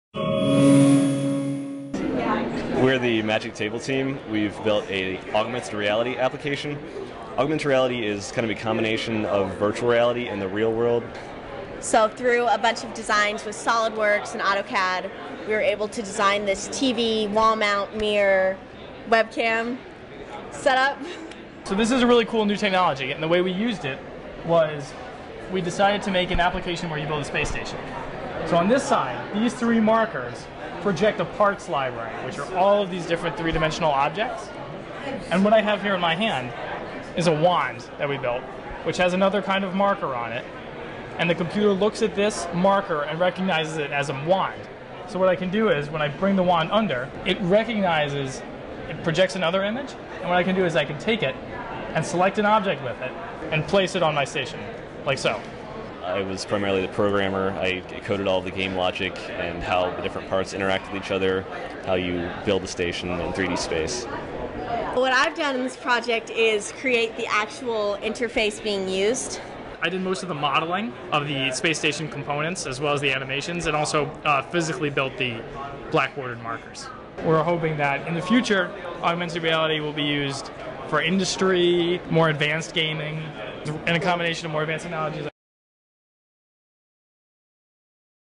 SPIRE-EIT REU Summer 2008: Magic Table
Overview of the Magic Table team's interactive augmented reality project during the Summer 2008 SPIRE-EIT Research Experience for Undergraduates held at Iowa State University's Virtual Reality Applications Center.